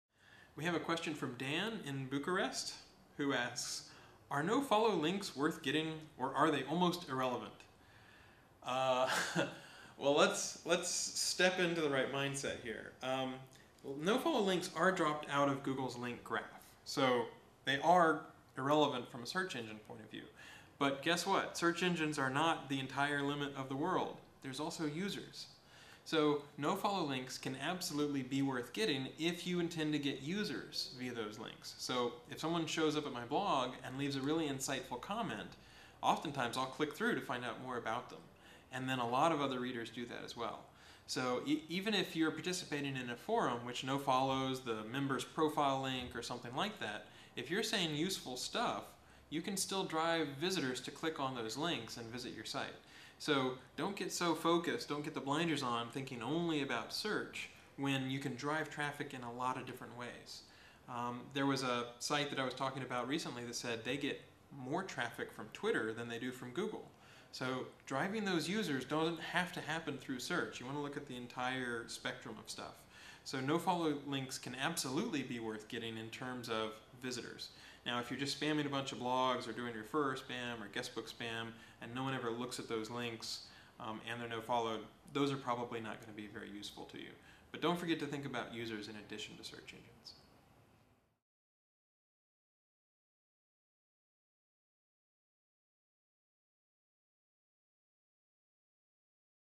Are nofollow links irrelevant?
Dan from Bucharest asks: "Are nofollow links worth getting? Or are they almost irrelevant?"